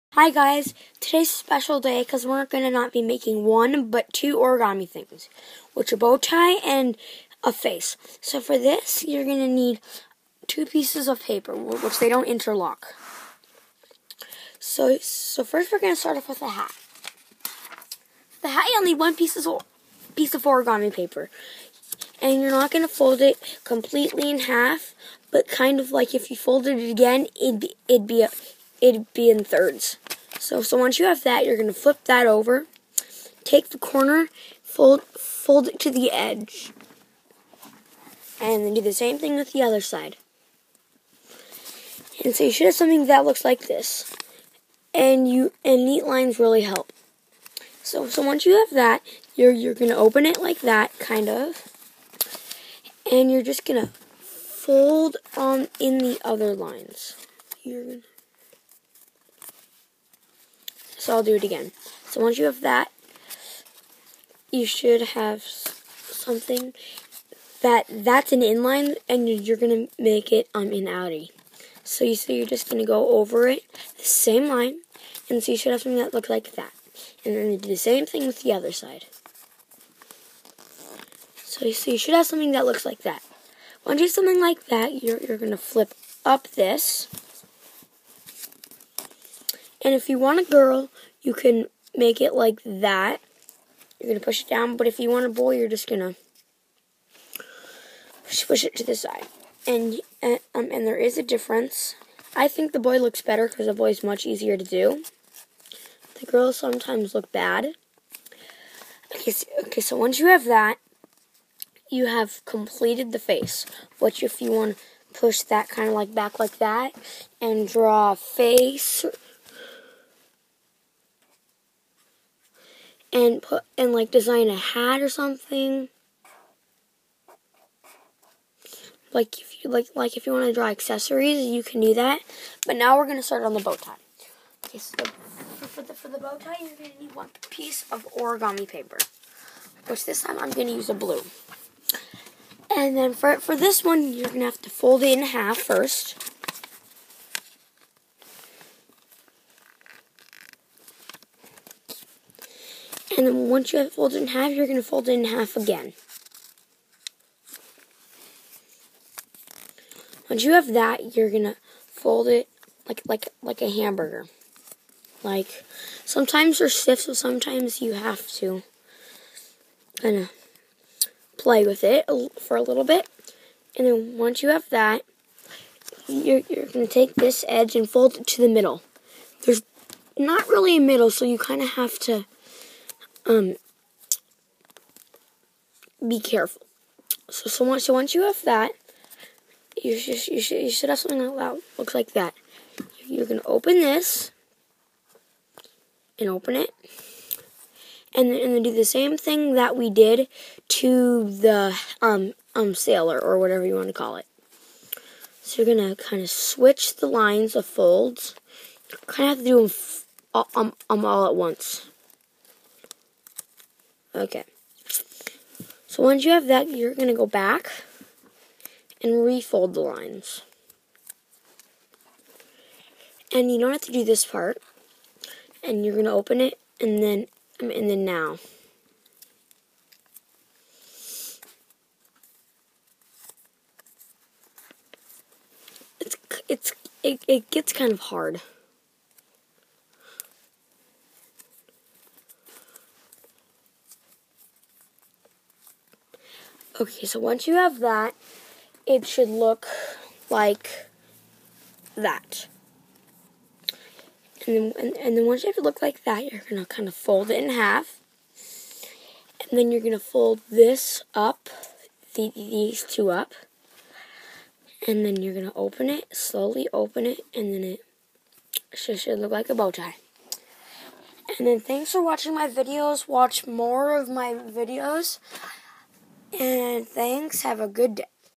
Origami bow tie and a sailor hat!
My video is about a sailor hat and a bow tie thanks I hope you like my movie thank you!!!